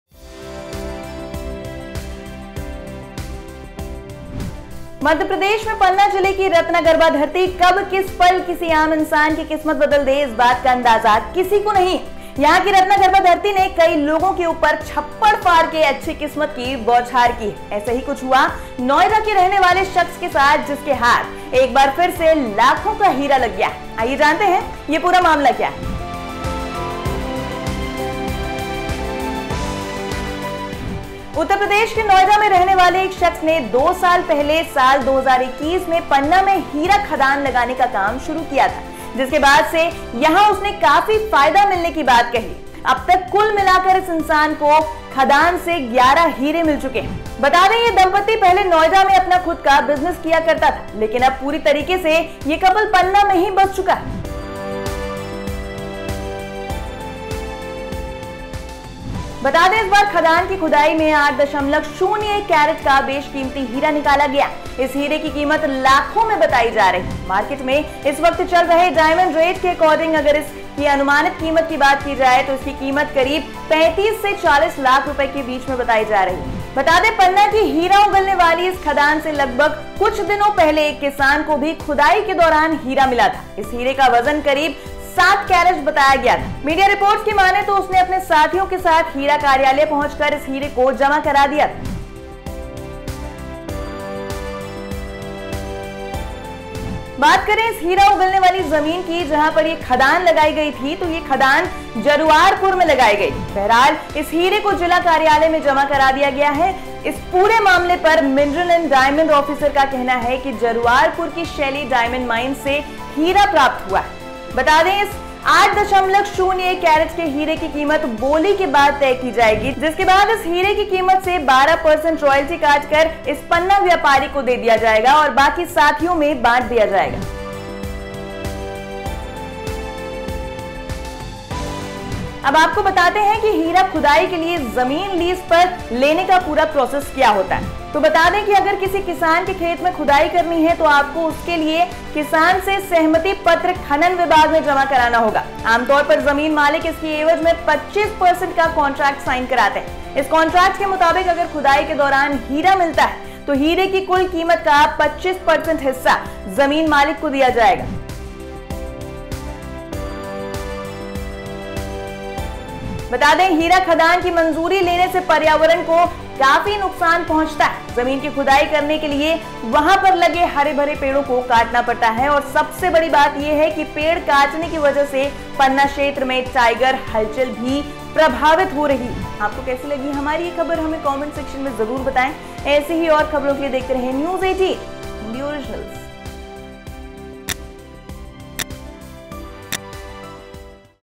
पन्ना में फिर मिला Diamond, इस शख्स की चमकी किस्मत!
मध्य प्रदेश (madhya pradesh) में पन्ना (panna) जिले की रत्नगर्भा की धरती कब किस पल किसी आम इंसान की किस्मत बदल दे इस बात का अंदाज़ा किसी को नहीं है..पन्ना जिले की रत्नगर्भा ज़मीन ऐसा बना दे इस बात का अंदाज़ा किसी को नहीं होता है. यहां की रत्नगर्भा धरती ने कई लोगों के ऊपर छप्पर फाड़ के अच्छी किस्मत की बौझार की है. ऐसा ही कुछ हुआ नोएडा के रहने वाले शख्स के साथ जिनके हाथ एक बार फिर से लाखों का हीरा (diamond)लगा है.